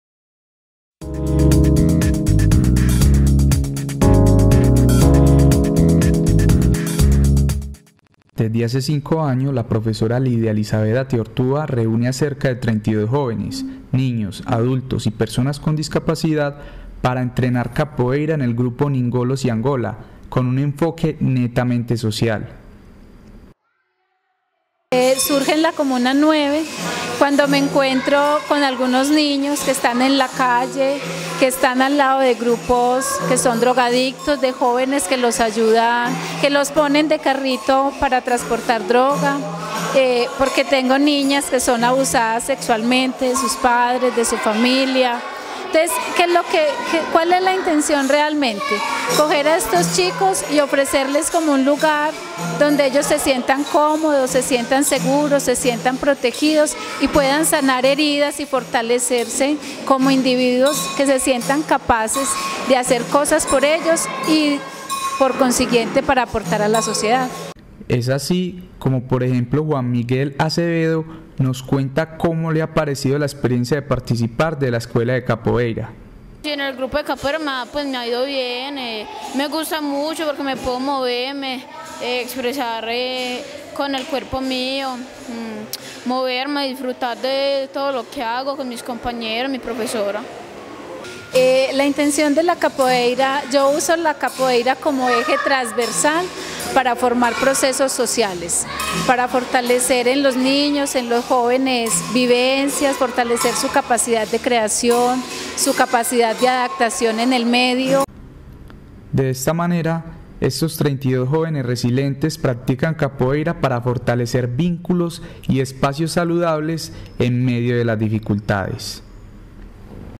Escuela de capoeira resiliente en Medellín
La profesora de Capoeira, Lidia Elizabeth Atehortúa, dirige un grupo de 32 jóvenes, adultos, niños y personas con discapacidad que en medio de las adversidades, se reúnen para crear vínculos y disfrutar de este deporte.